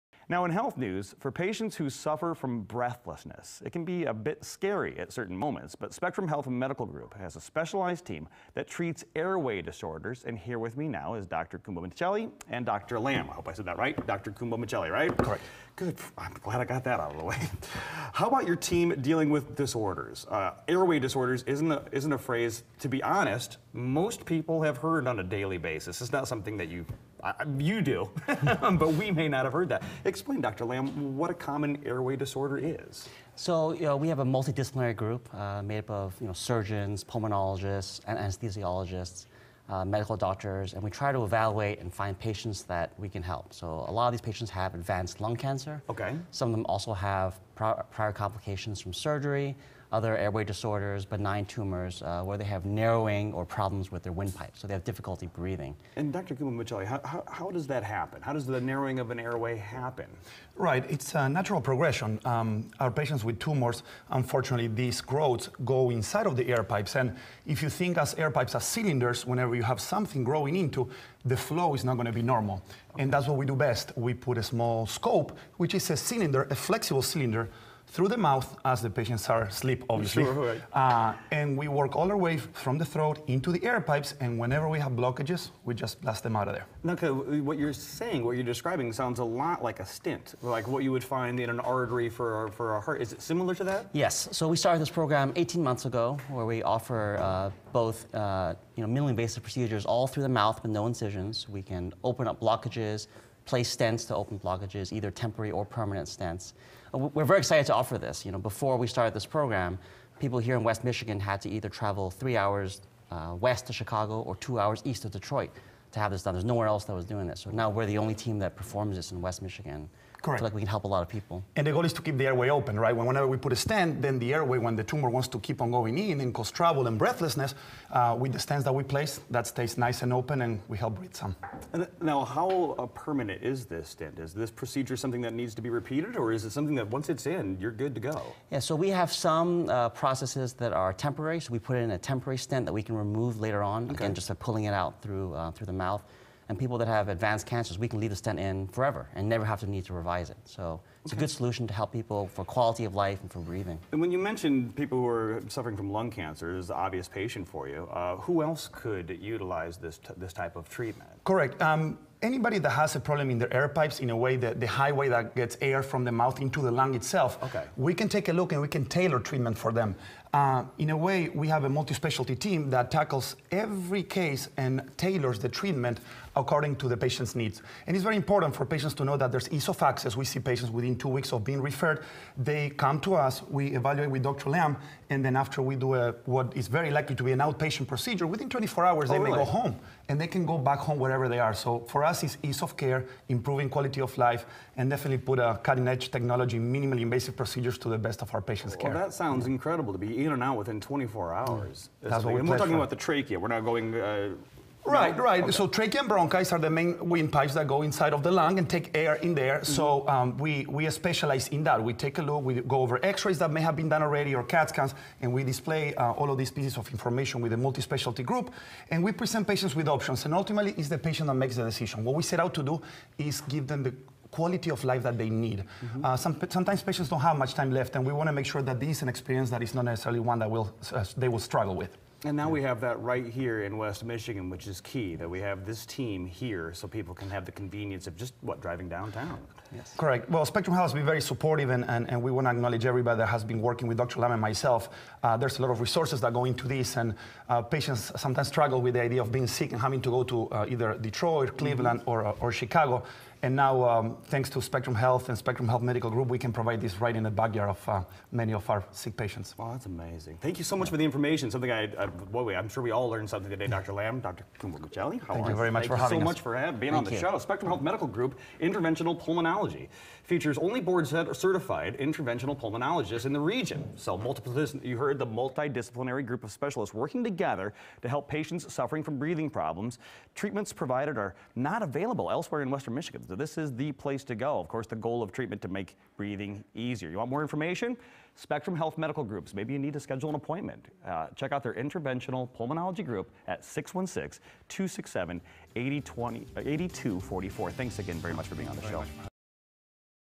Spectrum Health Interventional Pulmonary Group
Gustavo Cumbo-Nacheli, MD, and Geoffrey Lam, MD, discuss interventional pulmonology and its use to improve quality of life for people with airway disorders.